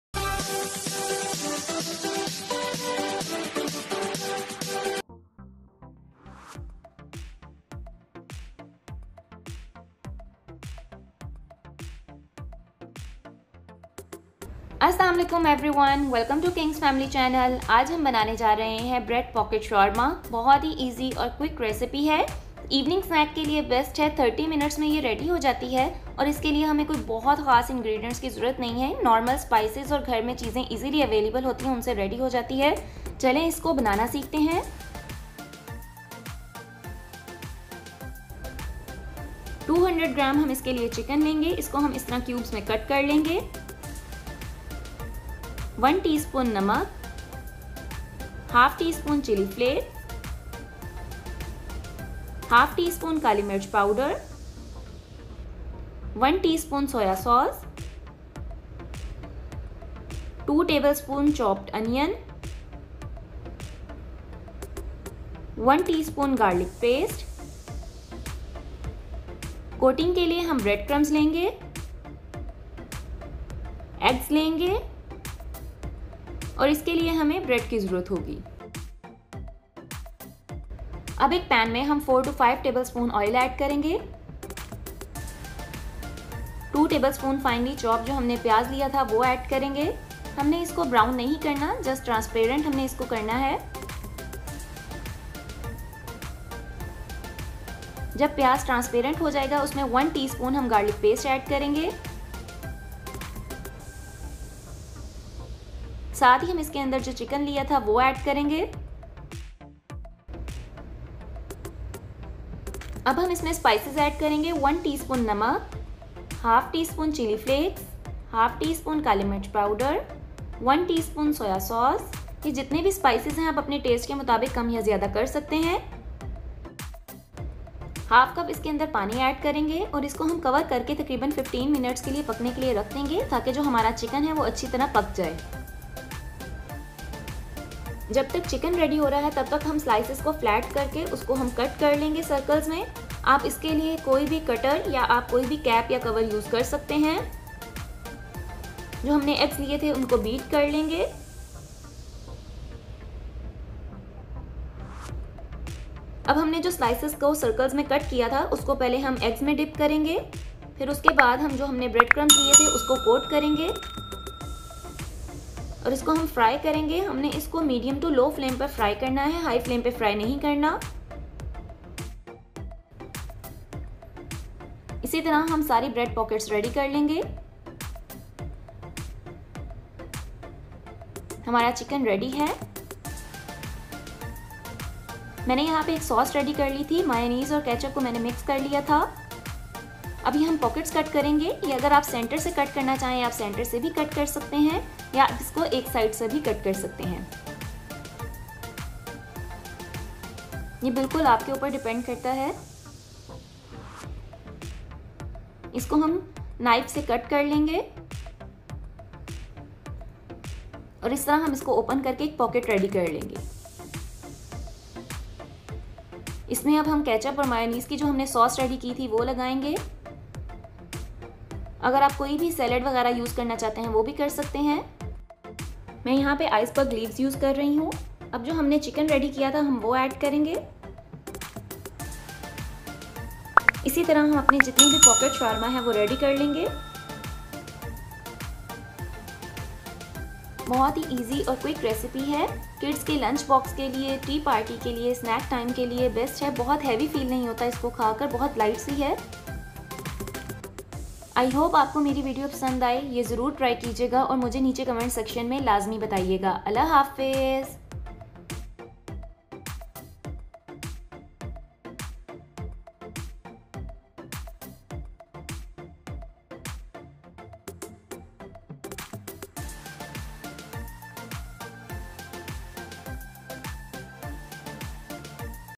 Bread pocket Shawarma/easy mini pocket Shawarma/ kids lunchbox snack idea /urdu/hindi
**Ingredients **

* Bread

* Eggs for coating

* Breadcrumbs for coating

* Chicken 200g

* Garlic Paste  1 teaspoon

* Onion chopped 2 tablespoon

* Red chilli flakes half teaspoon

* Black paper powder half teaspoon

* Oil